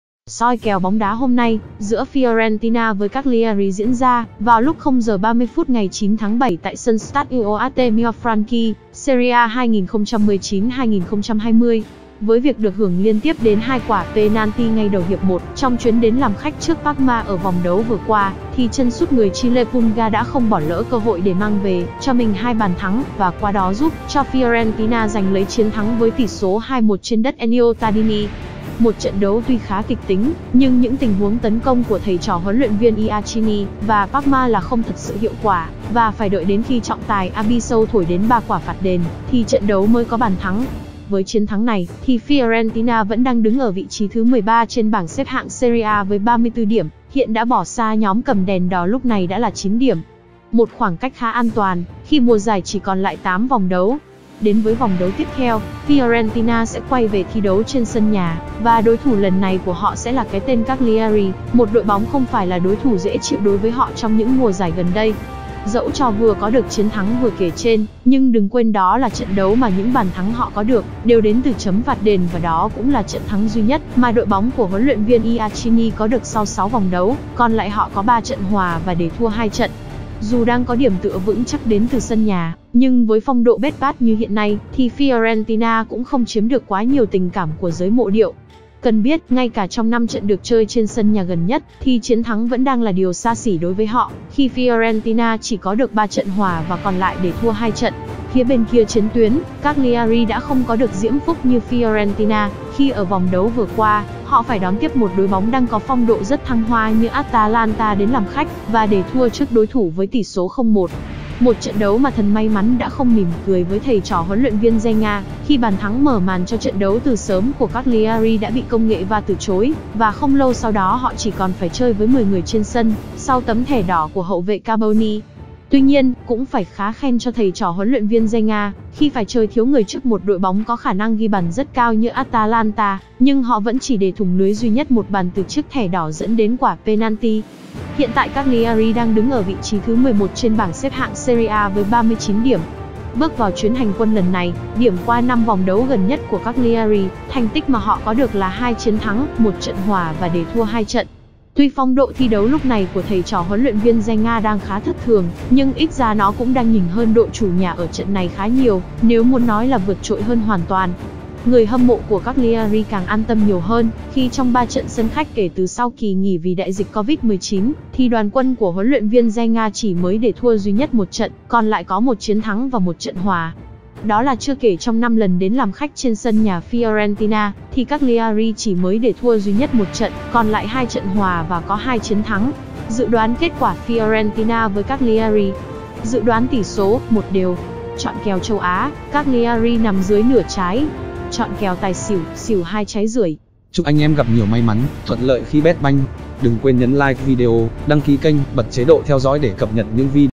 Fiorentina vs Cagliari 00h30 ngày 09/07 - Vòng 31 - Serie A 2019/2020
⚽️ Soi kèo bóng đá hôm nay Fiorentina vs Cagliari diễn ra vào lúc 00h30 ngày 09/07 tại sân Stadio Artemio Franchi, Serie A 2019/2020.
© Copyright by Phạt Góc Official ☞ Do not Re-up in any form.